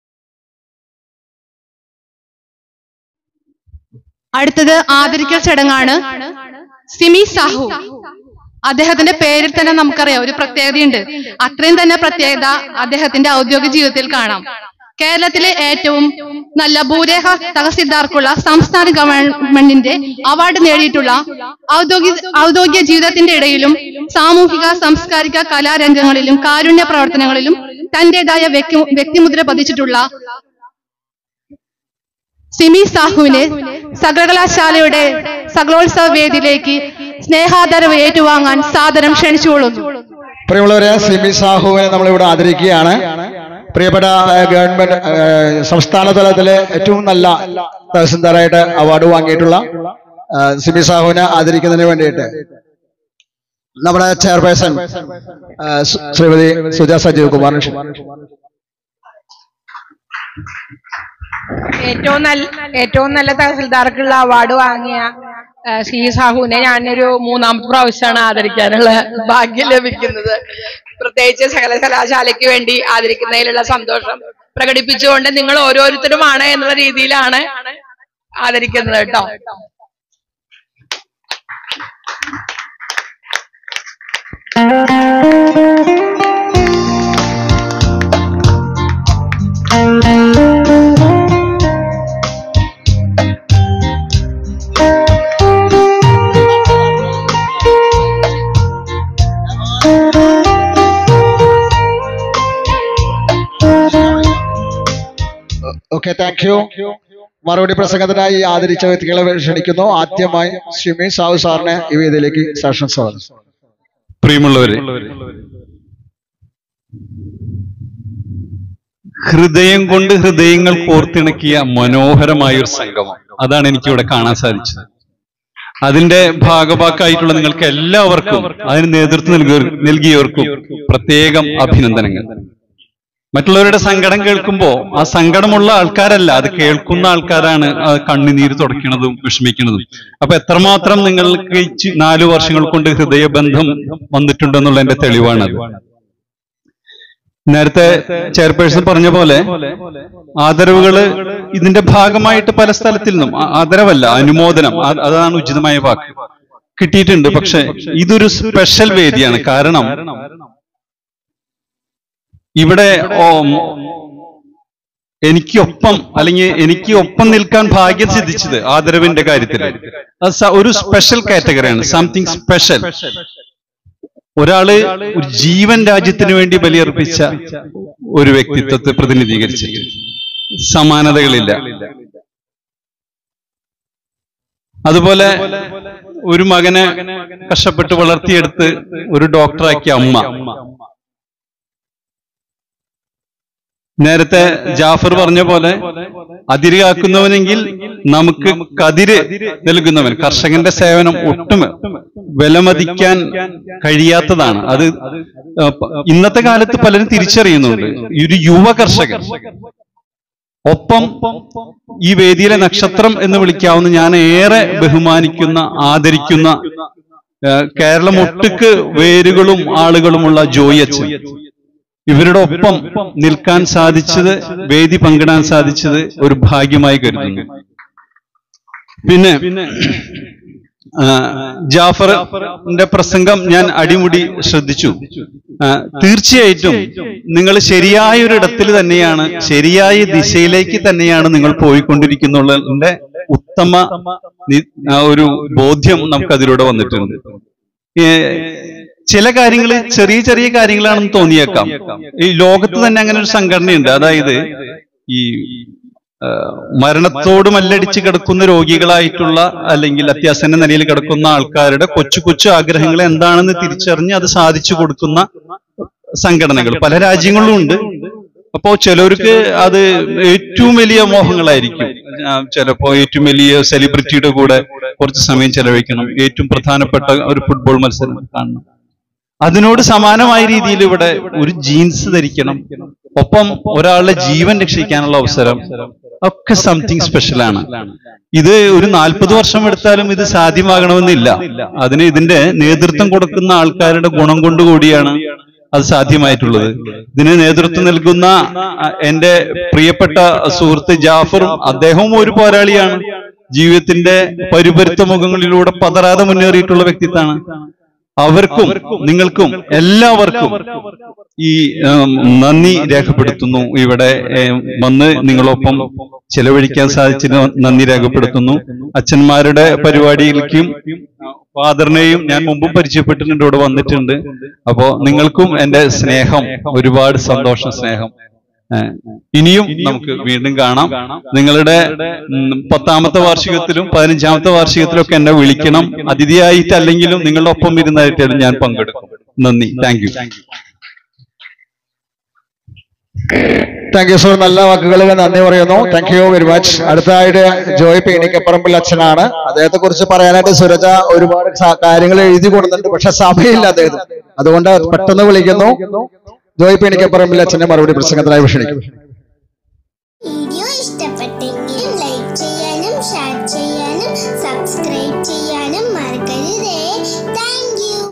മറുപടി പ്രസംഗം. അനുമോദനത്തിൻ്റെ മറുപടി പ്രസംഗം.
ar public media
മറുപടി പ്രസംഗം. ശ്രീ.സിമി സാഹു ( ഭൂരേഖാ തഹസിൽദാർ)
# മറുപടിപ്രസംഗം

സകലകലാശാല വാട്സാപ്പ് കൂട്ടായ്മയുടെ നാലാമത് വാർഷികം സകലകലോൽസവം'24.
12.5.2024 ഞായറാഴ്ച ഇരിഞ്ഞാലക്കുട ഹൗസ് ഓഫ് പ്രോവിഡൻസ് ഹോളിൽ നടന്ന പരിപാടിയിൽ സകലകലാശാലയുടെ അനുമോദനംഏറ്റുവാങ്ങി ശ്രീ: സിമി സാഹു( ദൂരേഖാ തഹസിൽദാർ)സംസാരിക്കുന്നു.
 വീഡിയോ ഇഷ്ടമായാൽ ലൈക് ചെയ്യുക.